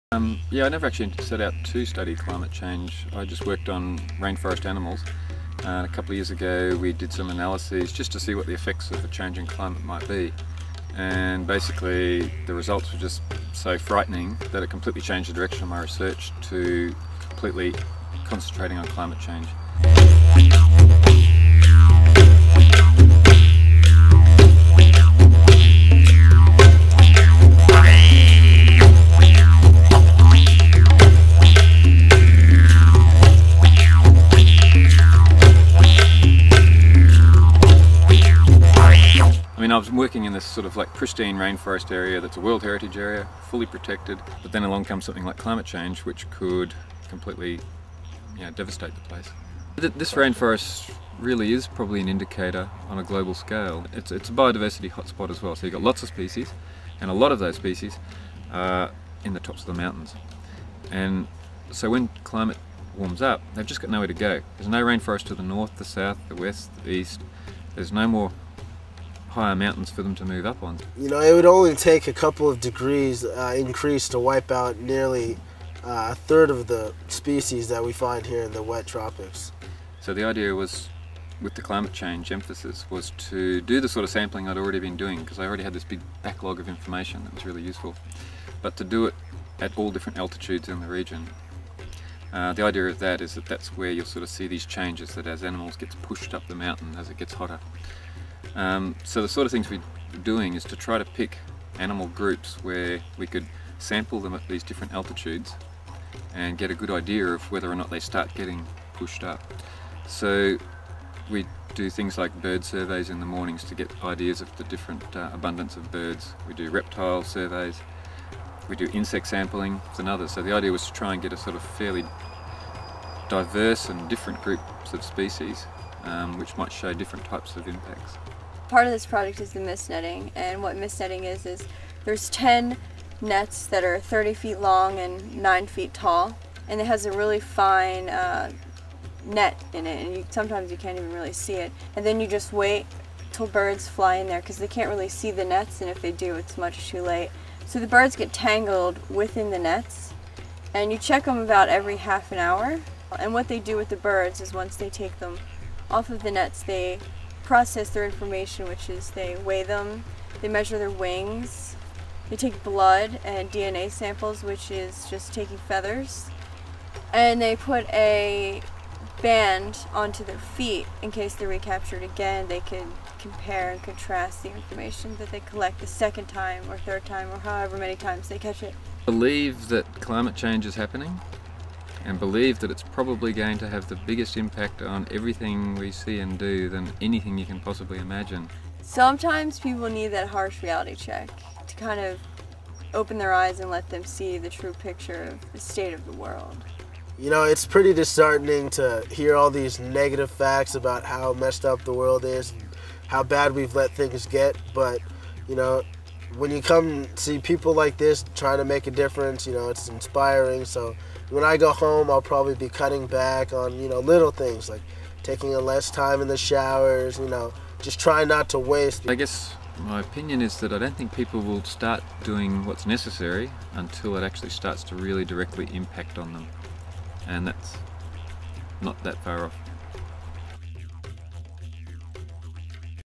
Climate Change Research in Australia
Earthwatch principle investigator Dr. Stephen Williams discusses how he came to study climate change in Australia's rainforest.